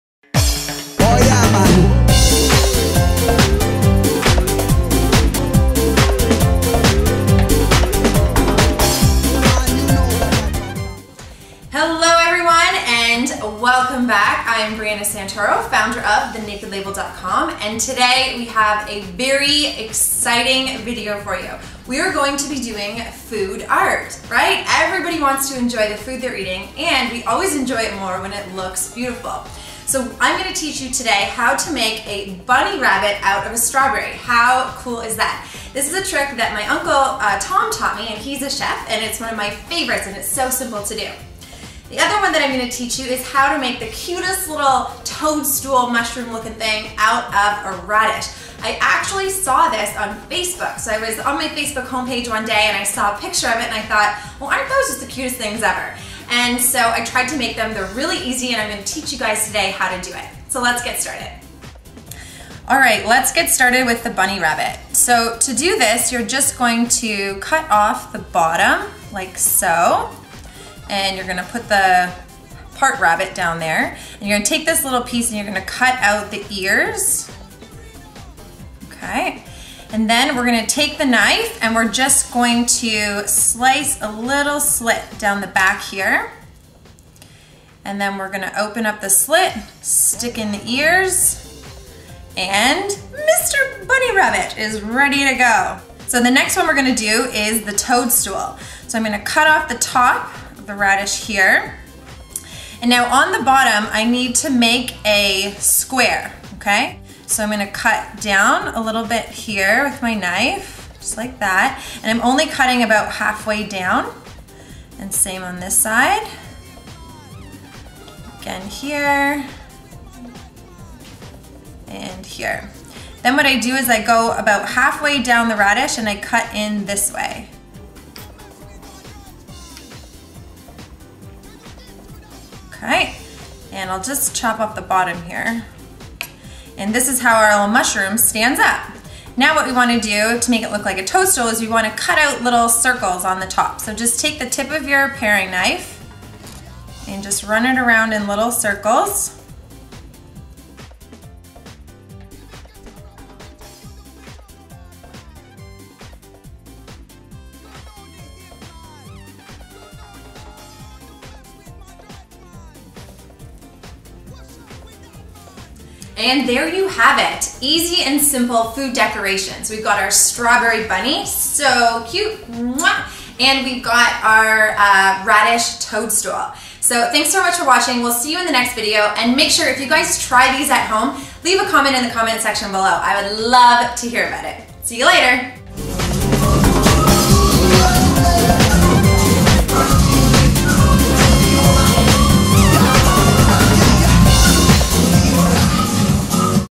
Food Art! Strawberry Bunnies and Radish Toadstools
Making food art - turning strawberries into bunnies and radishes into toadstools. Video presented by host Briana Santoro of The Naked Label, a website with great holistic nutrition tips and healthy recipes. These cuties are sure to spice up your dinner party and turn you into the talk of the neighbourhood!!